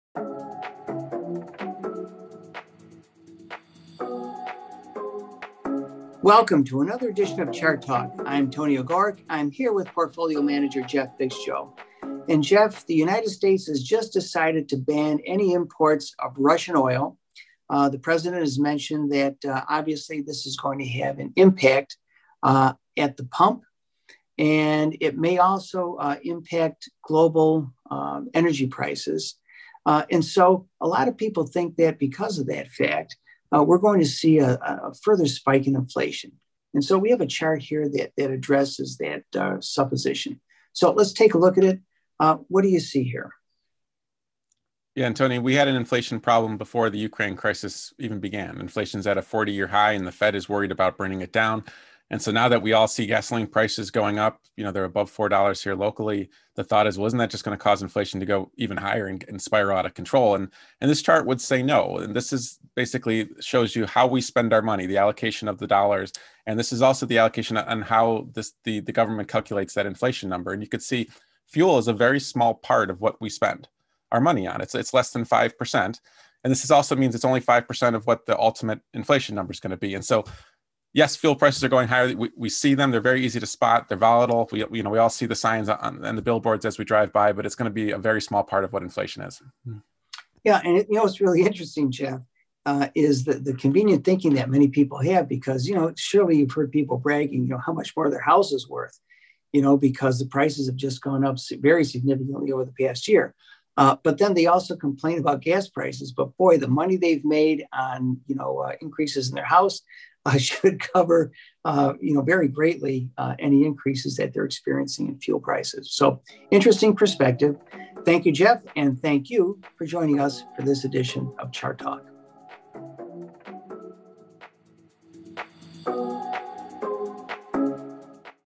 Why Fuel Prices Are Not Gassing Inflation
In this edition of Chart Talk, Tony Ogorek and Jeff Viksjo discuss how the Russian oil ban could be impacting inflation.